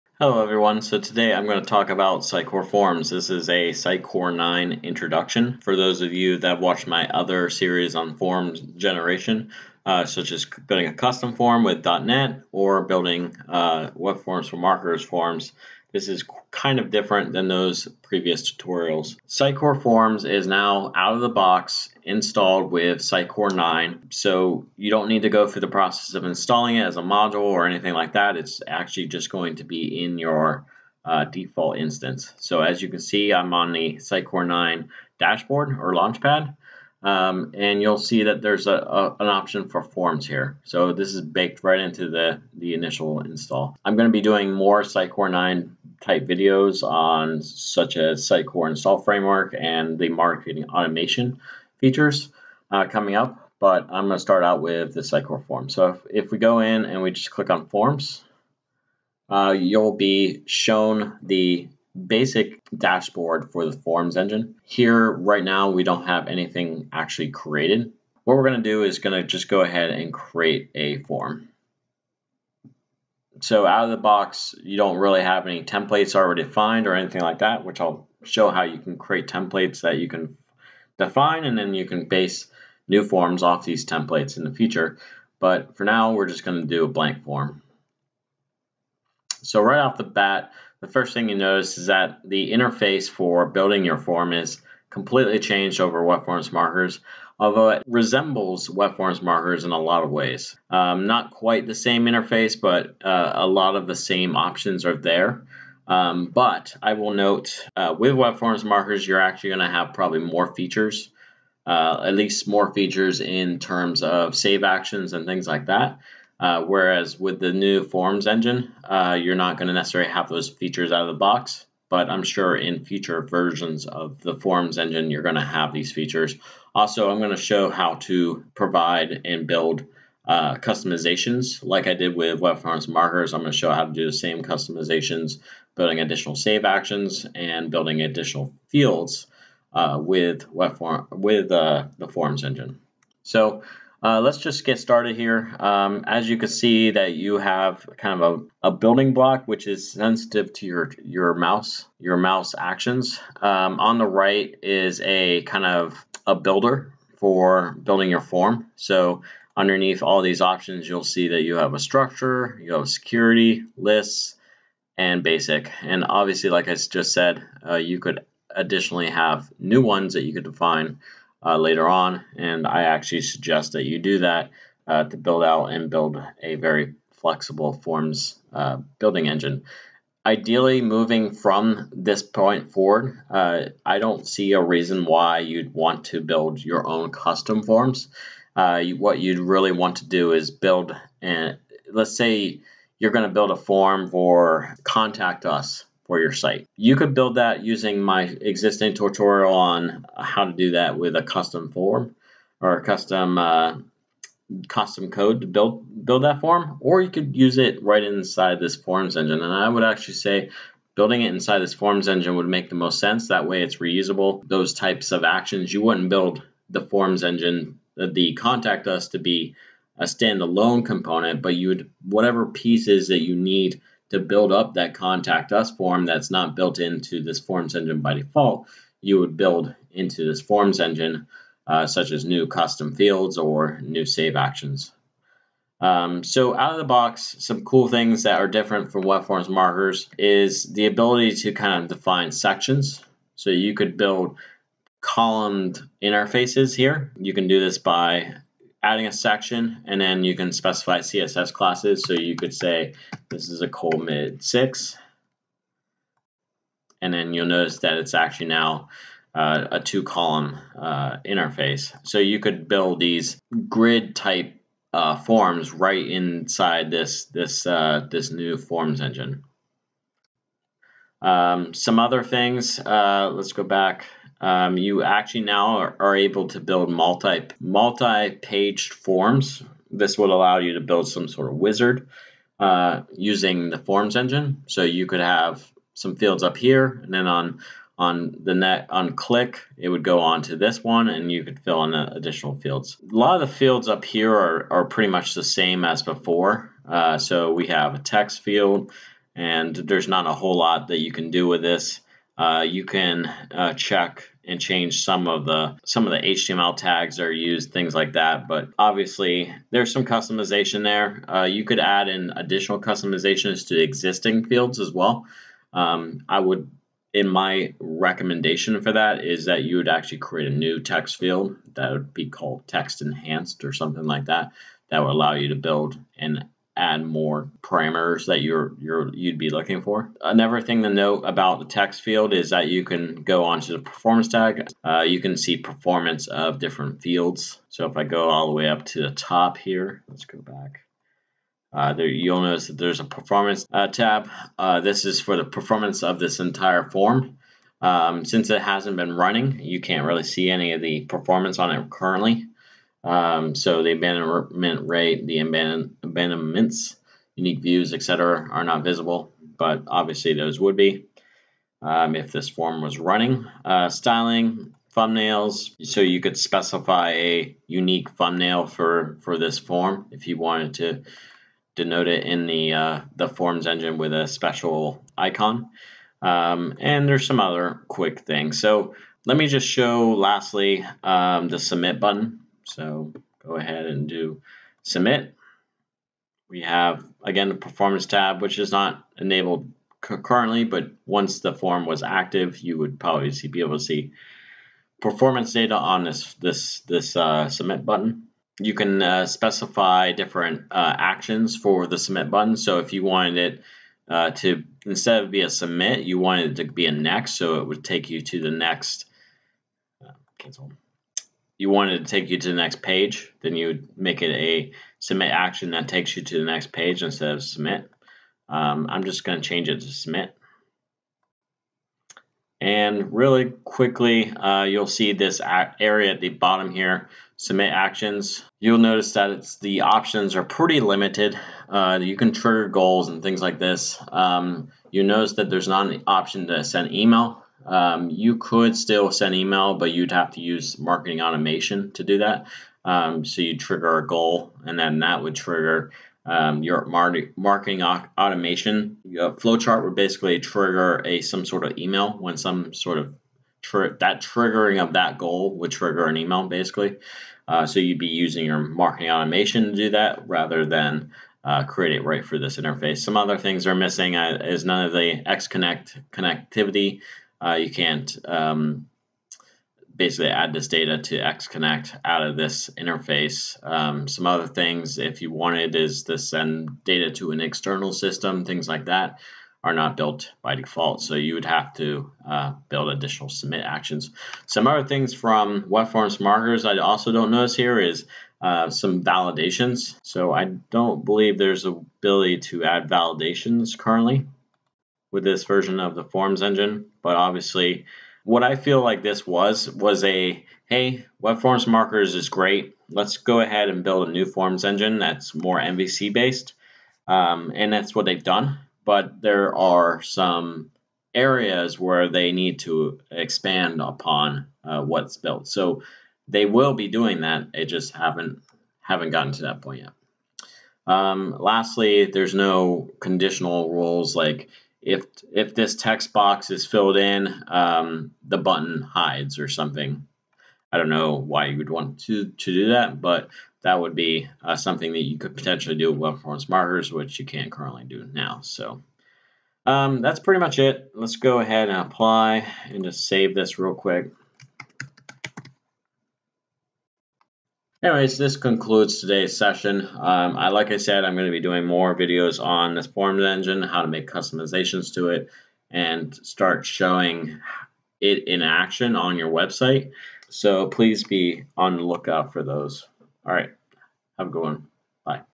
💻 Sitecore Forms in Sitecore 9 [ First Look ]
Introducing Sitecore Forms in Sitecore 9.  Sitecore includes forms out of the box.  In this video I introduce the basic building blocks of the new Sitecore Forms engine.